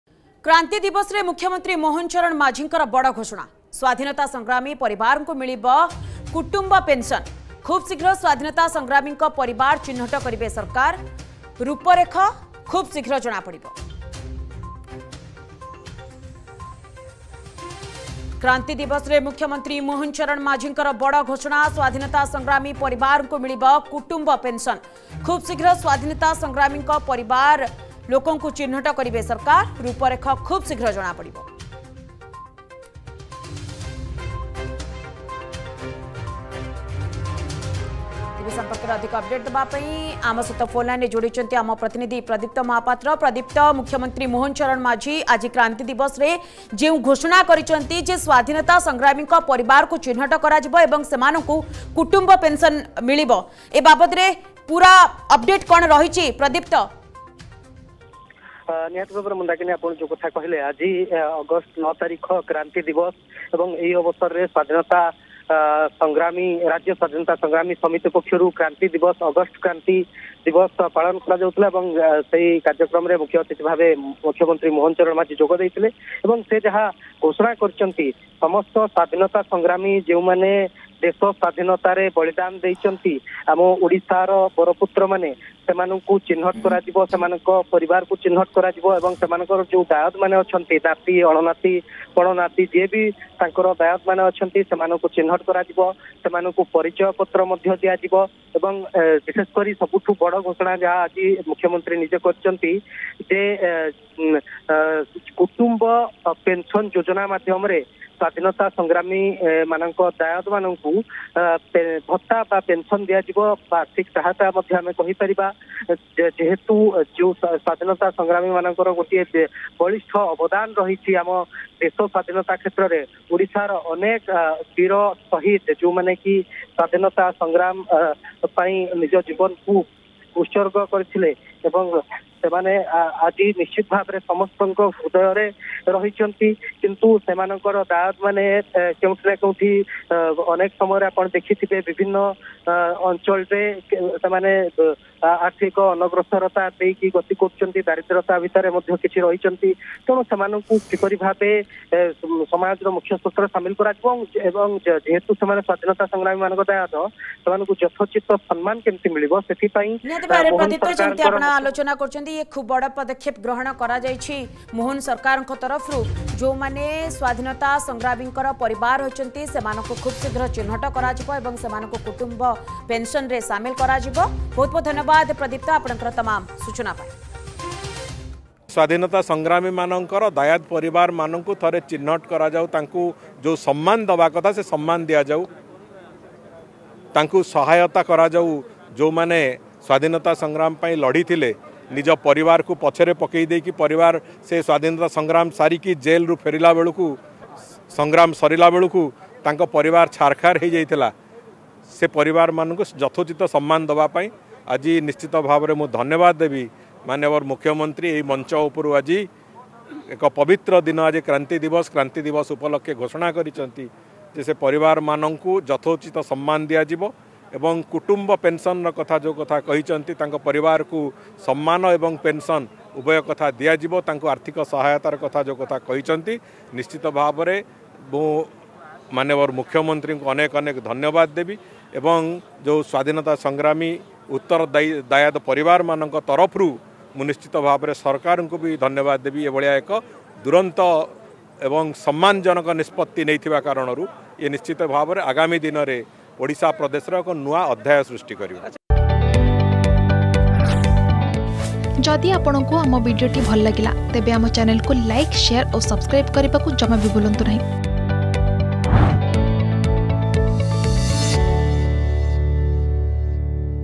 ସ୍ଵାଧୀନତା ସଂଗ୍ରାମୀ ପରିବାରଙ୍କୁ ମିଳିବ କୁଟୁମ୍ଵ ପେନ୍‌ସନ୍‌ || Kranti Diwas 2024
କ୍ରାନ୍ତି ଦିବସରେ ମୁଖ୍ୟମନ୍ତ୍ରୀ ମୋହନ ଚରଣ ମାଝୀଙ୍କ ବଡ଼ ଘୋଷଣା । ସ୍ଵାଧୀନତା ସଂଗ୍ରାମୀ ପରିବାରଙ୍କୁ ମିଳିବ କୁଟୁମ୍ଵ ପେନ୍‌ସନ୍‌ ।